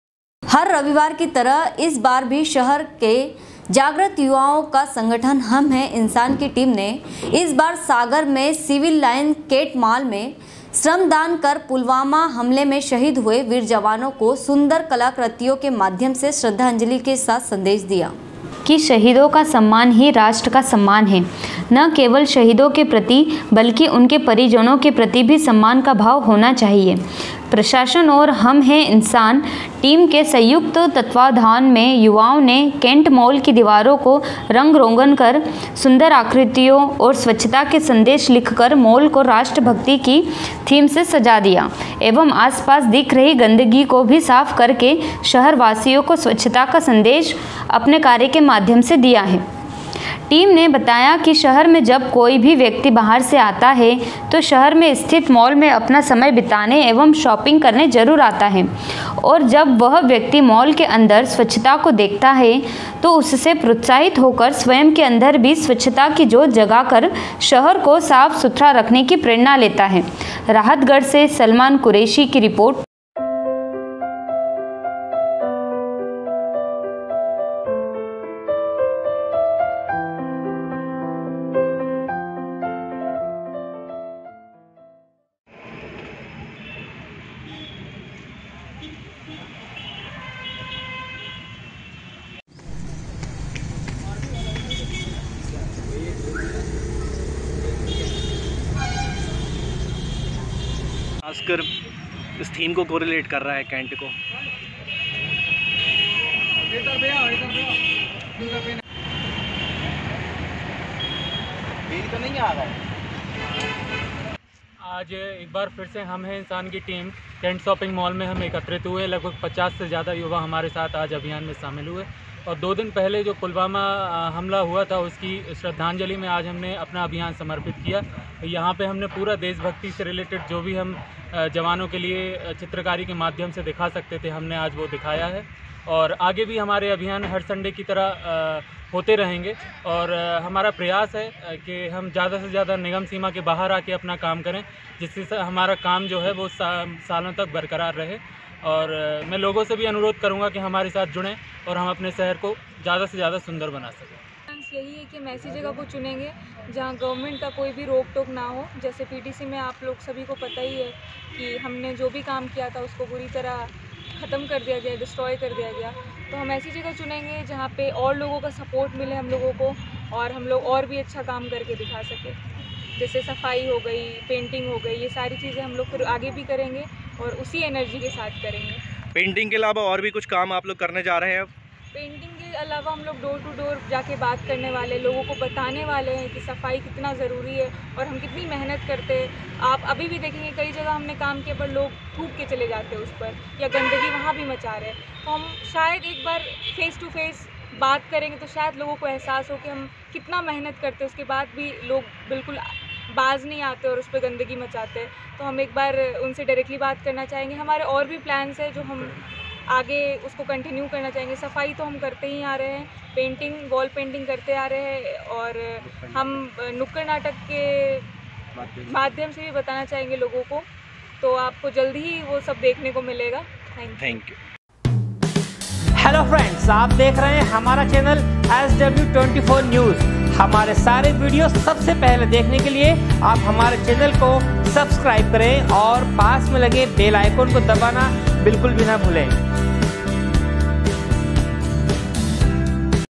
sw24news rahatgarh, 19.02.2020
हर रविवार की तरह इस बार भी शहर के जाग्रत युवाओं का संगठन हम हैं इंसान की टीम ने इस बार सागर में सिविल लाइन कैंट माल में श्रृमदान कर पुलवामा हमले में शहीद हुए वीर जवानों को सुंदर कलाकृतियों के माध्यम से श्रृद्धांजलि के साथ संदेश दिया राहतगढ़ से सलमान कुरेशी की रिपोर्ट#sw24news